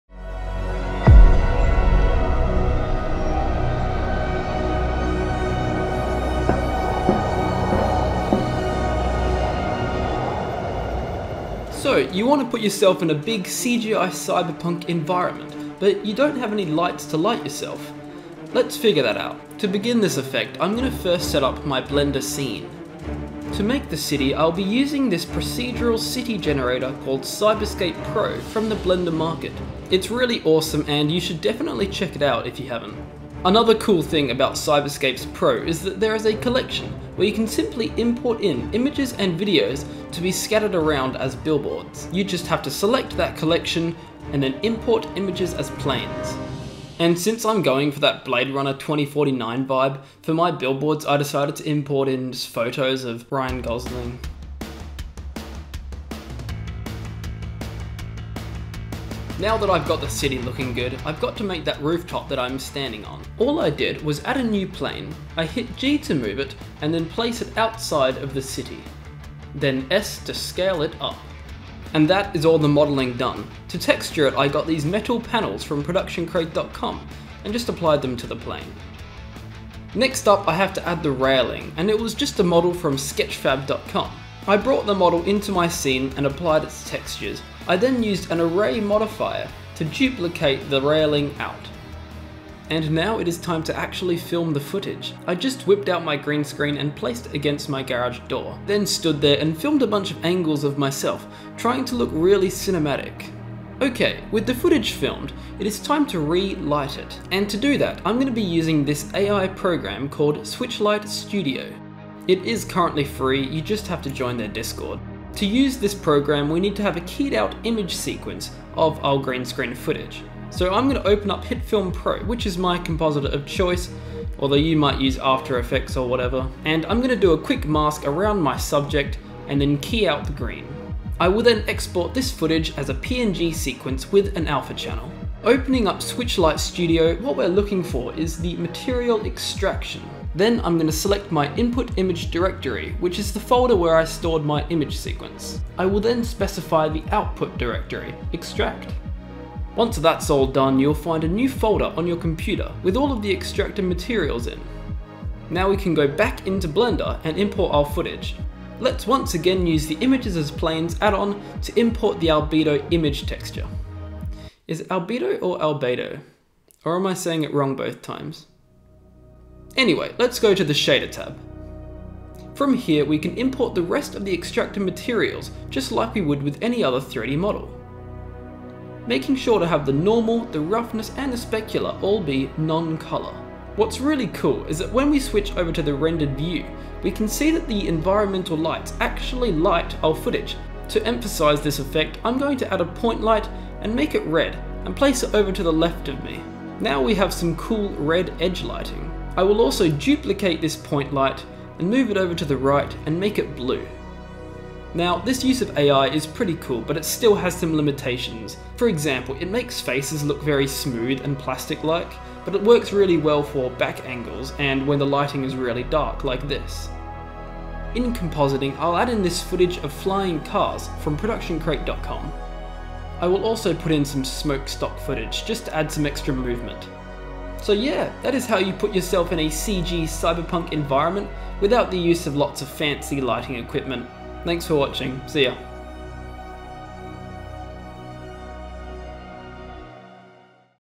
Put Yourself in Any CGI Environment - BLENDER (Episode 2)
How to Put live action footage into a CGI Environment in Blender Part 2.
Hello,
Today I'm creating this Cyberpunk style Neon city scene, and I will be showcasing how I put myself (live action Greenscreen footage) into this CGI environment. This video also shows how you can use AI program SwitchLight Studio to relight your live action footage.
Inspired by Cyberpunk 2077, Blade runner 2049 and Altered Carbon.

Big thanks to Ryan Gosling!

Timestamps:
0:00 - Intro
0:19 - Blender Scene
1:32 - Filming Footage
1:45 - Ai Relighting / SwitchLight Studio
3:24 - Adding details